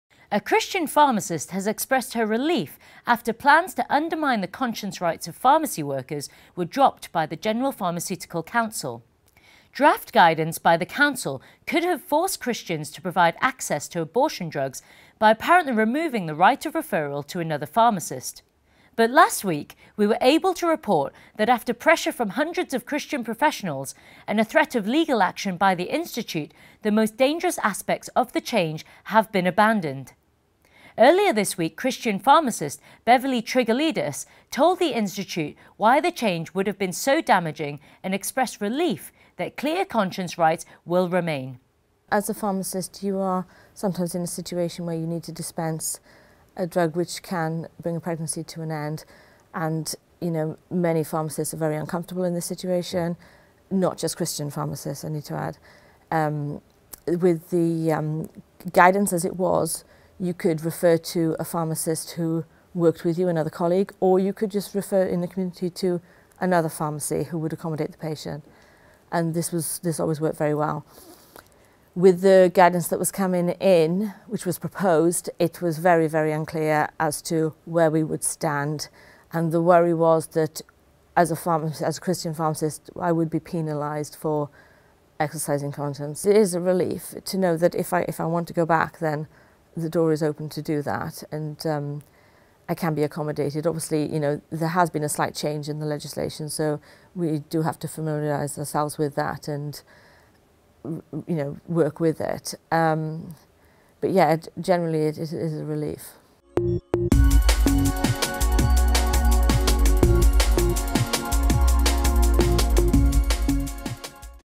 Relief as dangerous pharmacy plans scrapped
A Christian pharmacist has expressed her relief after plans to undermine the conscience rights of pharmacy workers were dropped by the General Pharmaceutical Council.